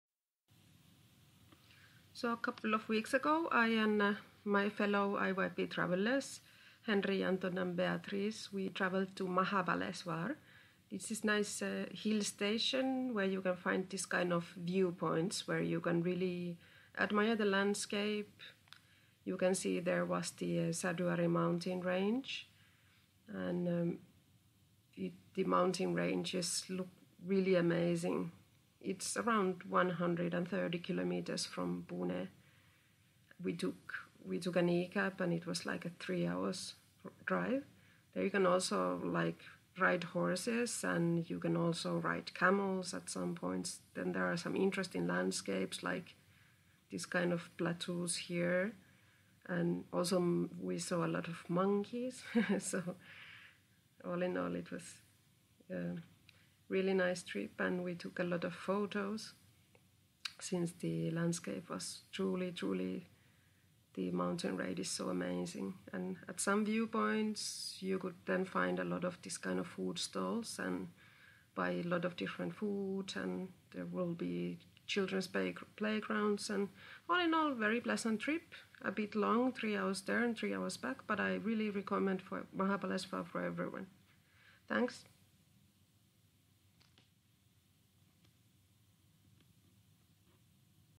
IEP - Week 17 - Inka
One Sunday we took an e-cab and traveled to Mahabaleshwar with Henri, Anton and Beatrice. It’s a hill station located in the Sahyadri mountain range, a popular weekend destination for tourists from Pune and Mumbai. You can either drive or walk between the different viewpoints and enjoy amazing landscapes, great food (especially strawberries, for which the area is well known) and entertainment like games and boat cruises.